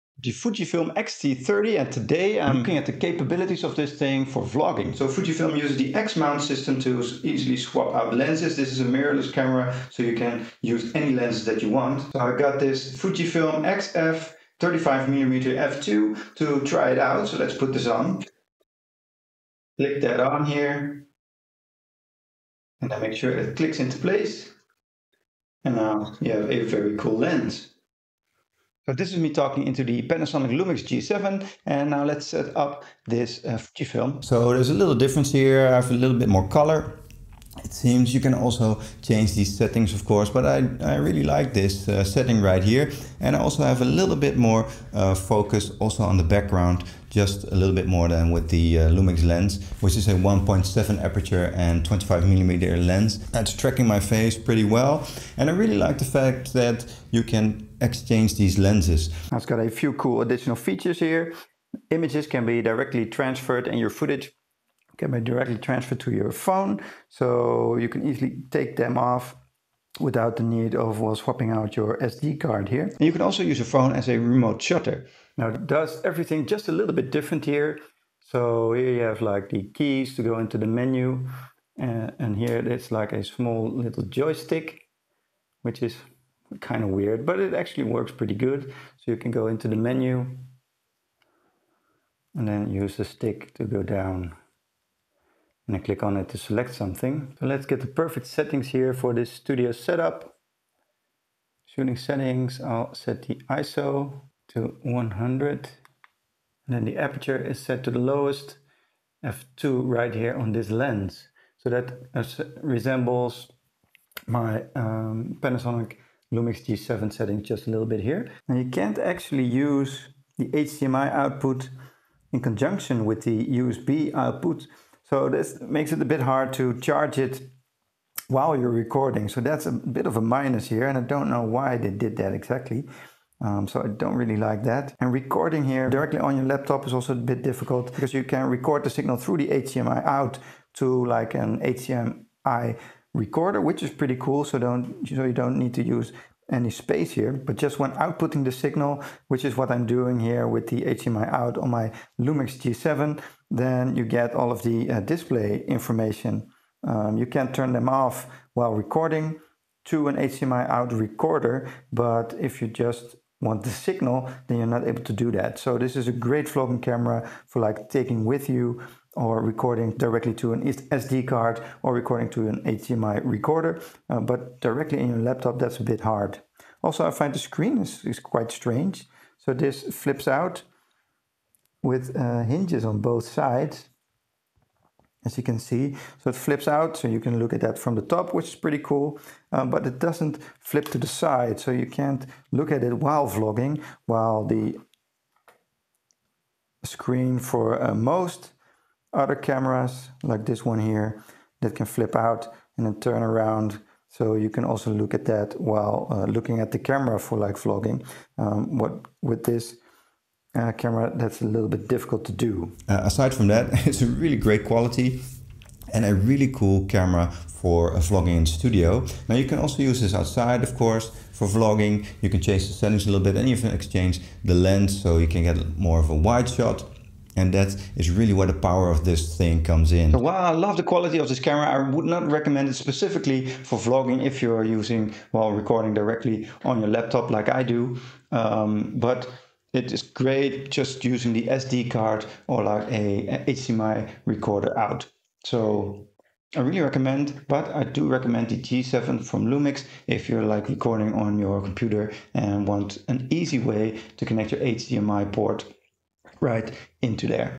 Best For Vlogging? Fujifilm X-T30 vs Panasonic Lumix G7
In this video, I'll review and compare the Fujifilm X-T30 vs Panasonic Lumix G7 for vlogging.